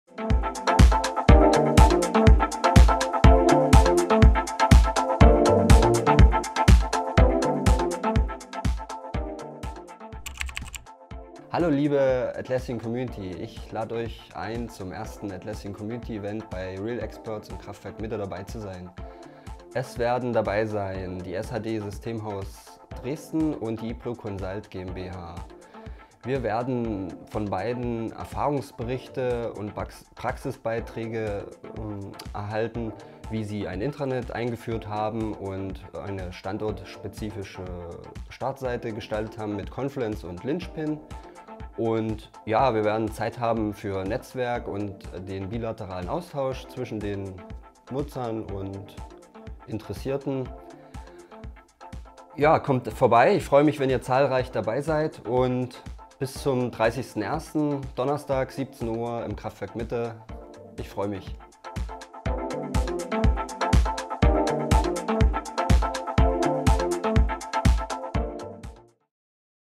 Teaser: Atlassian Community Event (User Group Meetup)
Am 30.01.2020 findet das erste Atlassian Community Event in Sachsen bei uns statt! Worum es dabei genau gehen soll und welche Themen diskutiert werden erklärt euch unser Experte Enrico Skottnik im Teaser.